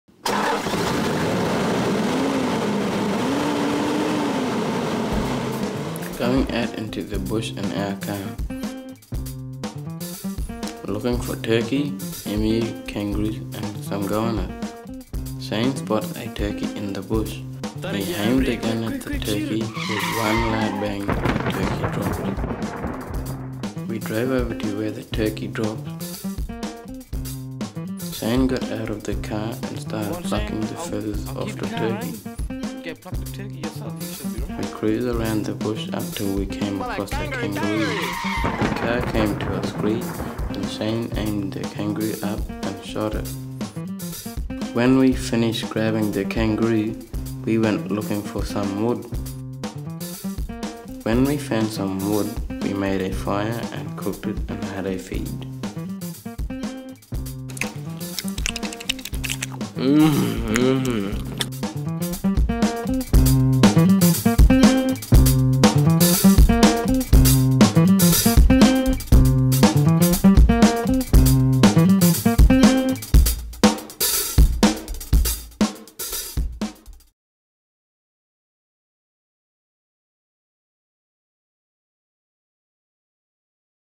MMR2005 Onslow Going Hunting
A popular activity with the youth of Onslow is 'going bush' to hunt for Bush Turkey, Kangaroo and Goanna, and this was the basis for this film. The students built a set, created clay figures and recorded voice-overs and dialogue for this short animation.